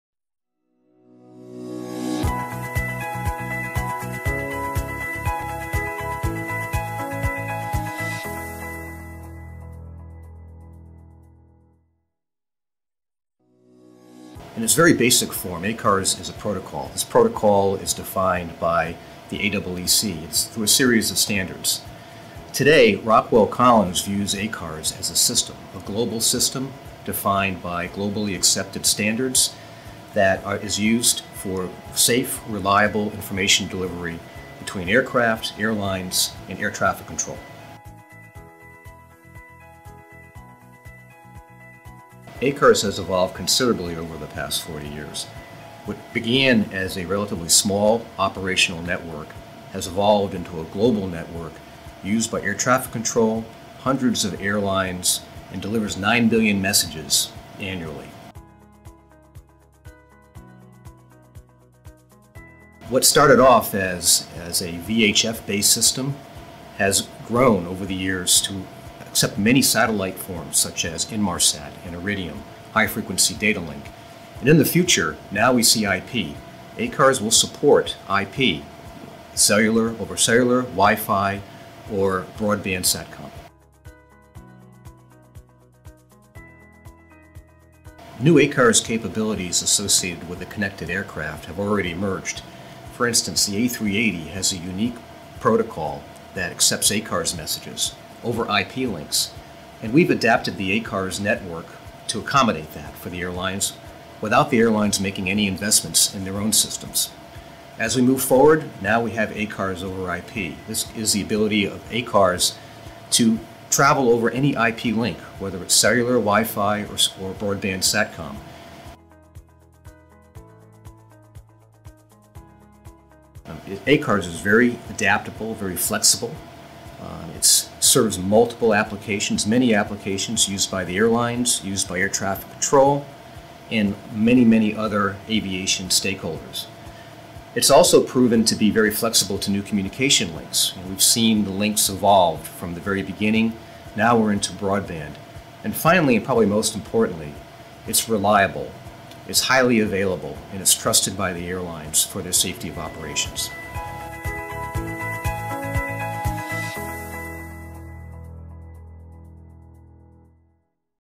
3 Reasons Why ACARS Is Here To Stay Now And Into The Future
Connected Aviation Today sat down with Dan Pendergast of Rockwell Collins to dig deeper into ACARS, the impact it’s made on aviation, and what the future holds.